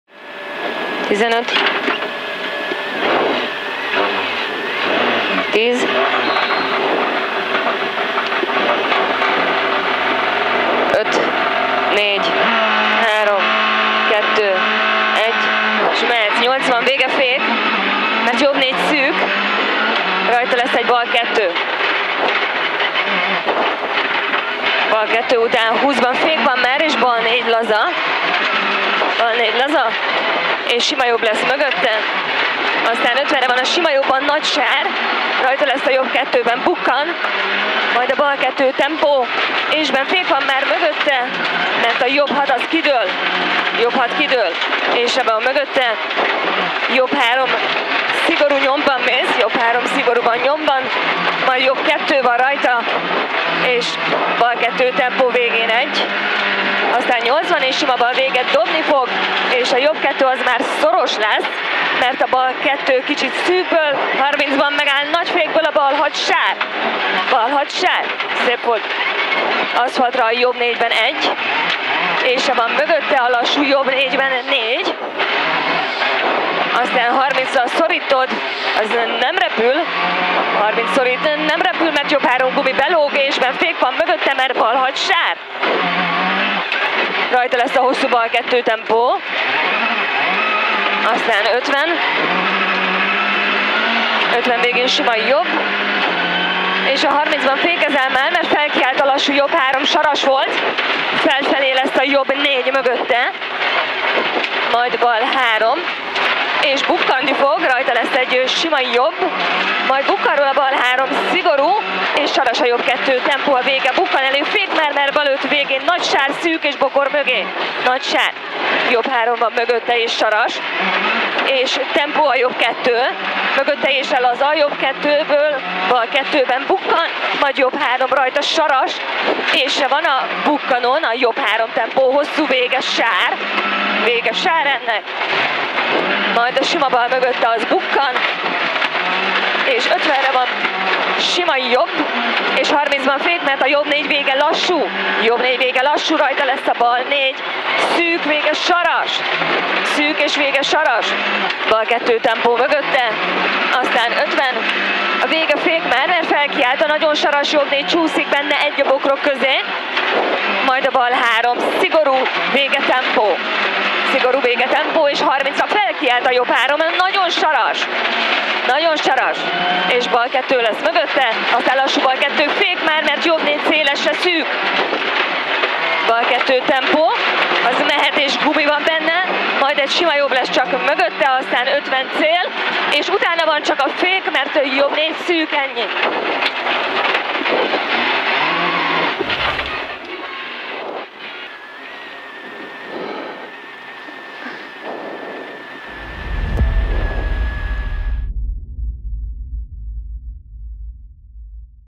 Veszprém Rallye 2023. - Kvalifikáció
Izgalmas belső kamerás felvétel Turán Frici és Farnadi Ági kvalifikációs futamáról a Veszprém Rallye-n.